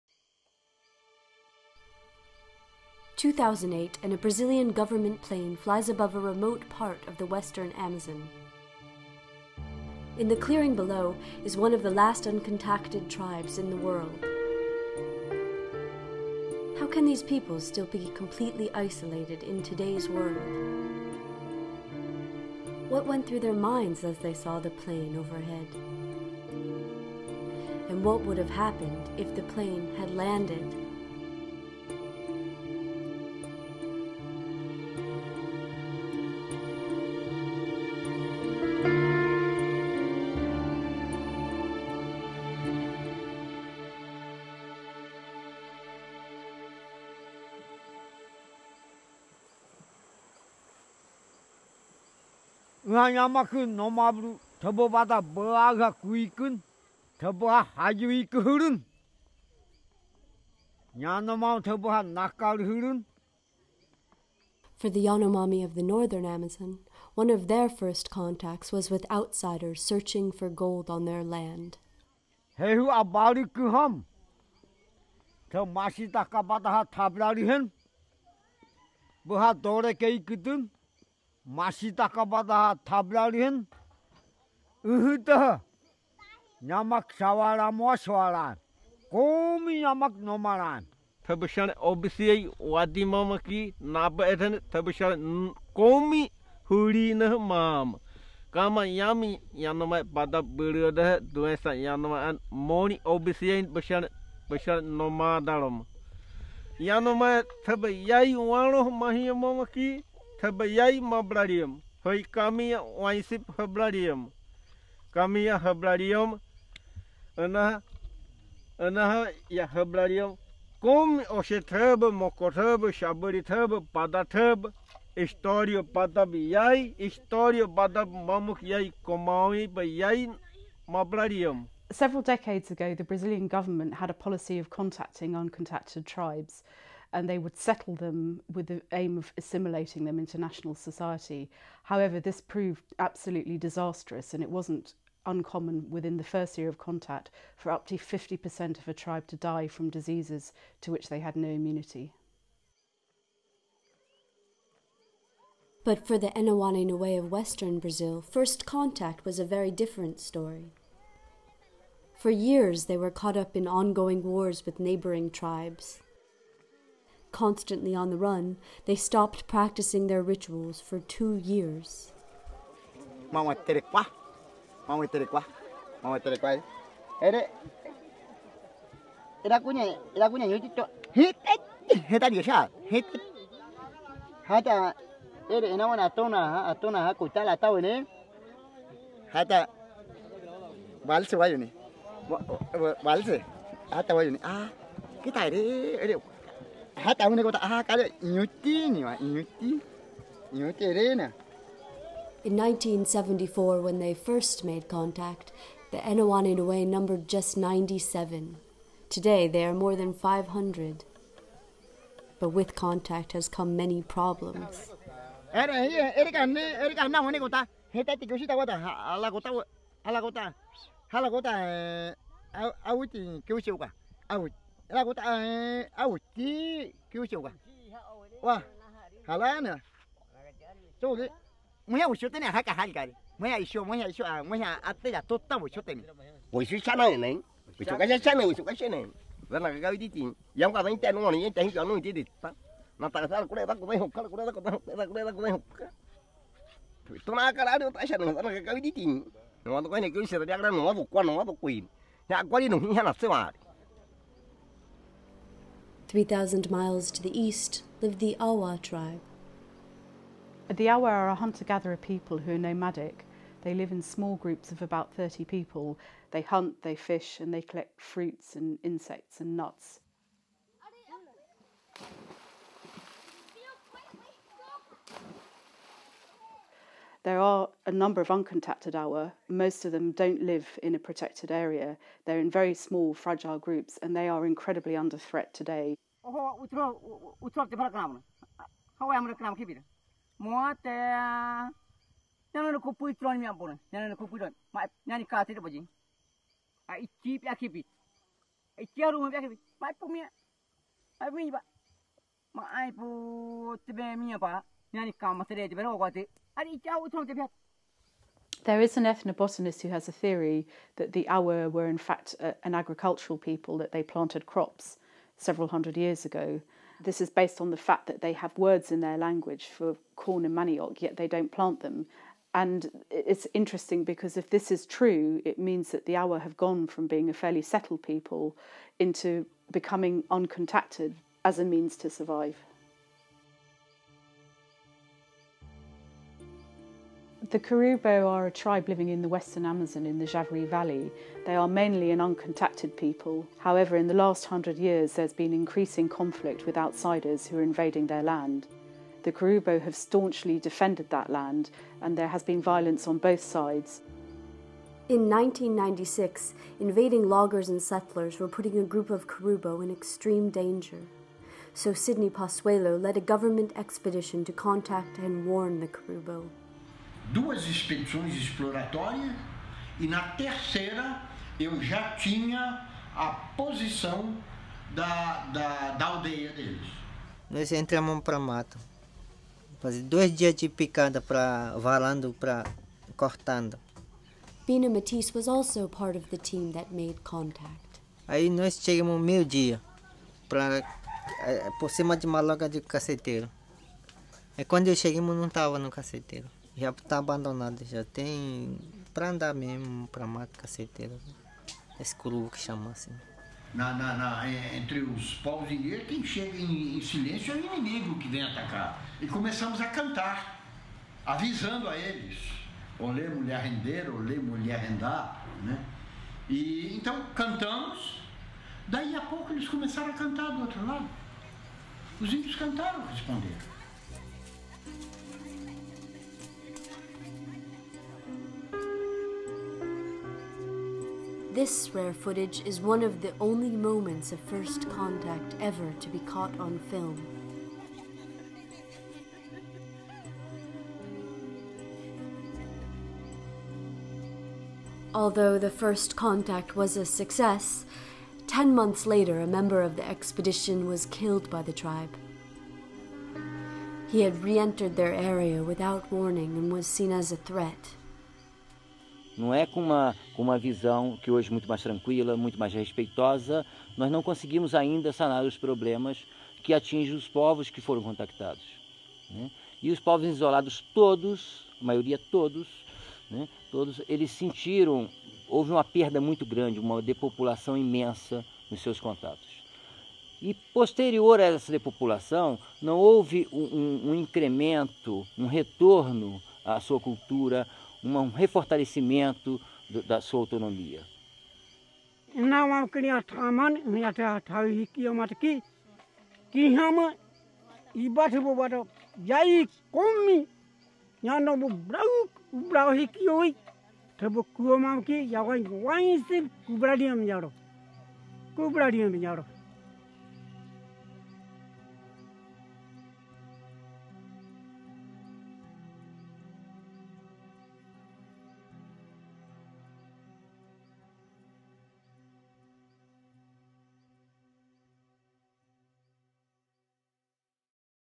Un extraño en la selva
Un avión sobrevuela las tierras donde habita una tribu no contactada. ¿Qué habría pasado si el avión hubiese aterrizado?

El episodio nos permite dar la palabra a algunos pueblos indígenas de Brasil para que hablen sobre sus experiencias del primer contacto y de los peligros que vinieron a continuación. La historia demuestra que el contacto forzado ha traído enfermedades, muerte, deforestación y pérdida de sus hogares ancestrales en la selva.

Los indígenas aislados dependen por completo de su selva para sobrevivir. Si sus tierras no se protegen pronto, serán arrasadas por madereros, ganaderos, proyectos extractivos y otras actividades económicas que se benefician del robo de sus tierras.

Ayúdanos a protegerlos y poner fin a una de las crisis humanitarias más trágicas y urgentes de nuestro tiempo